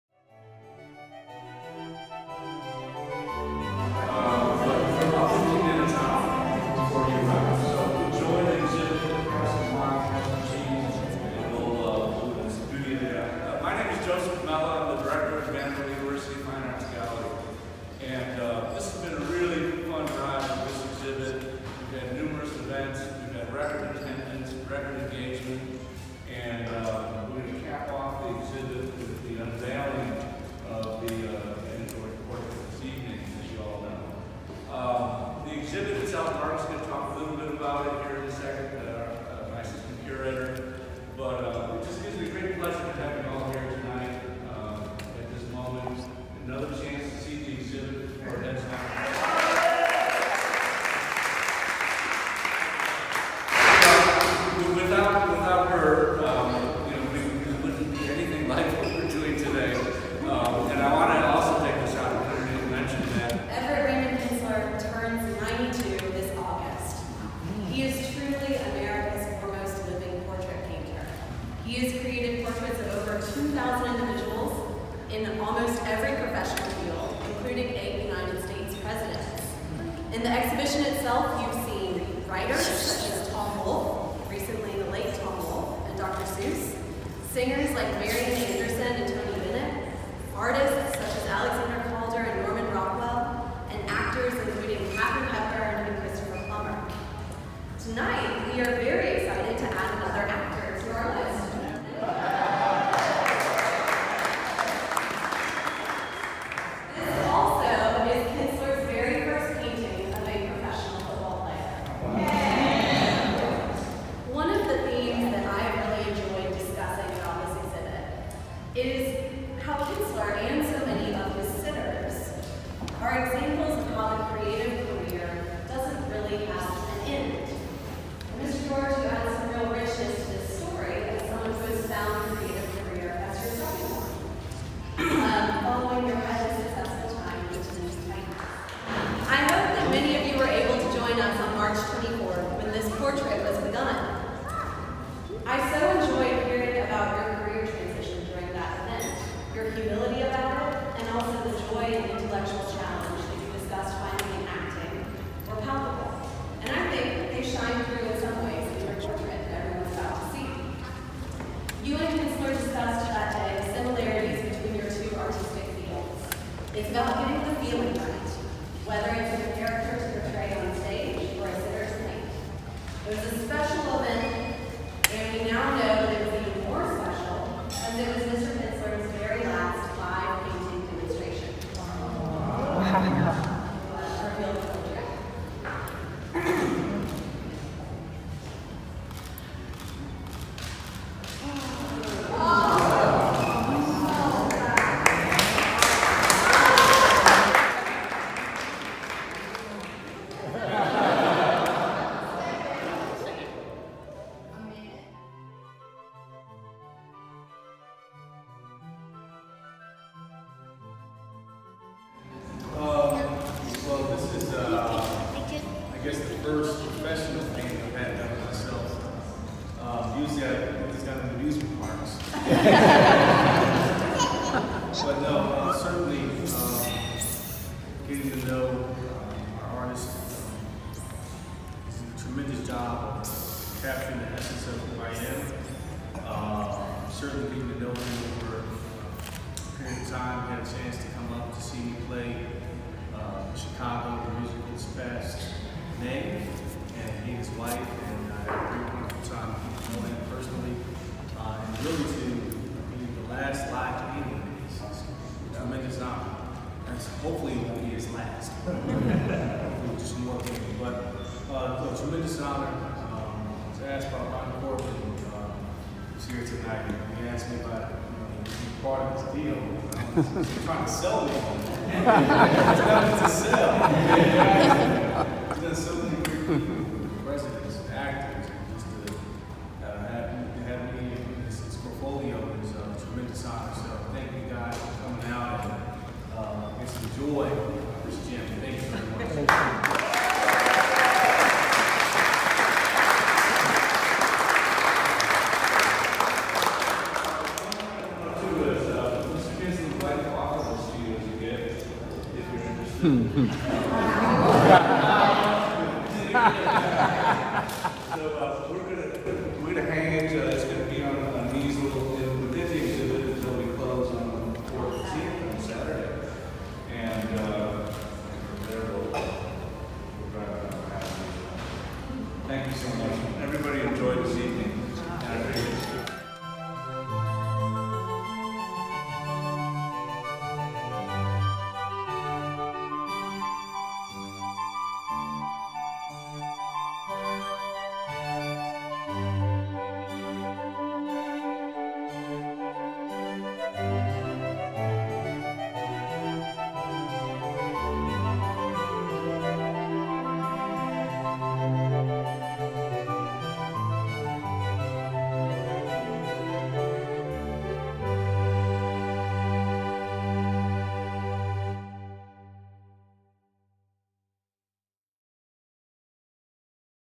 Kinstler's Portrait Unveiled
Everett Raymond Kinstler PORTRAIT UNVEILING of Eddie George at Vanderbilt Fine Art Gallery in Nashville.  AMazing exhibit of Kinstlers work for the past few months and Mr. Kinstler's LAST LIVE DEMO.  Wonderful!!